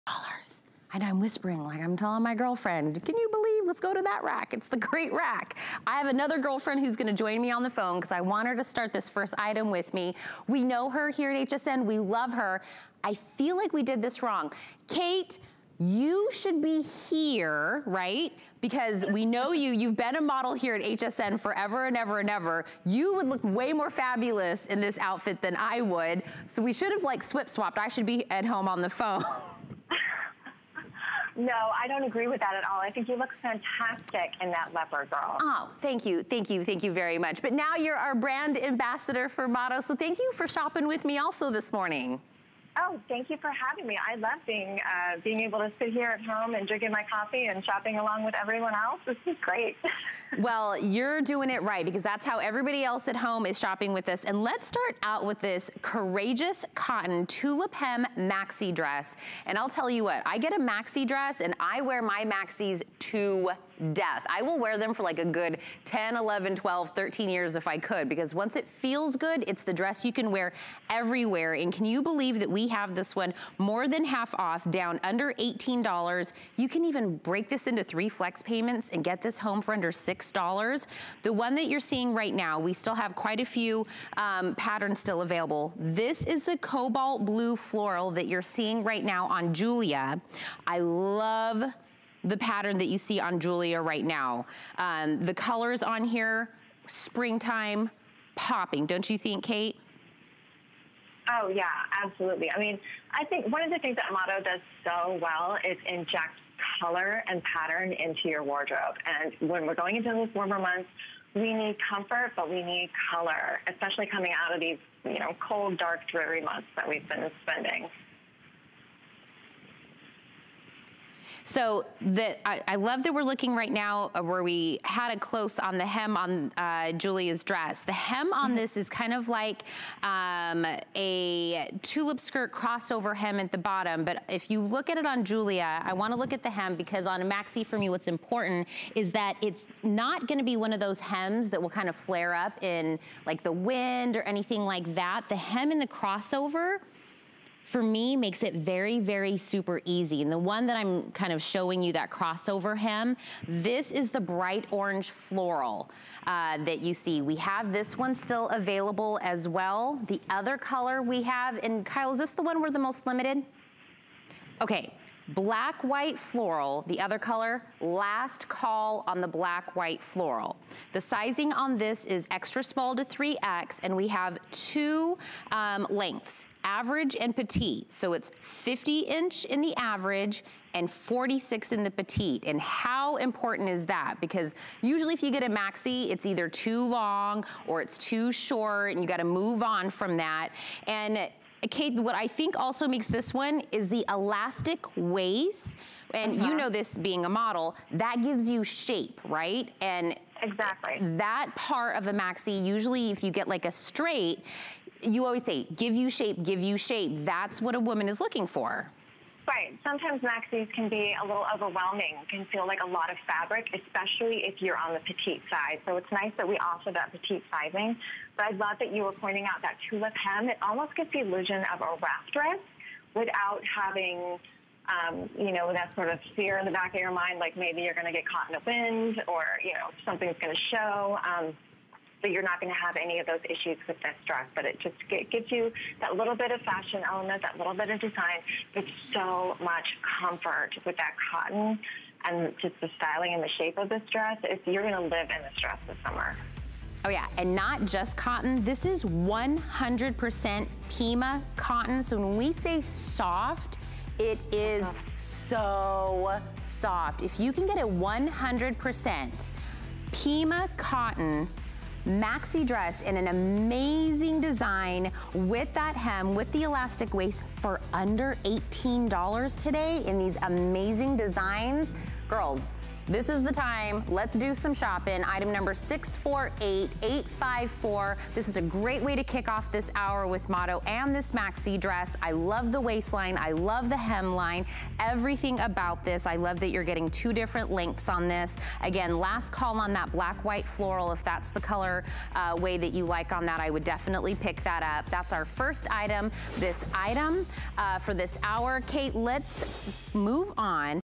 Motto Courageous Cotton TulipHem Maxi Dress
We've caught tulip fever and are swooning over this pretty dress. It's a classy maxi with a ladylike silhouette, casual yet appropriate for so many occasions.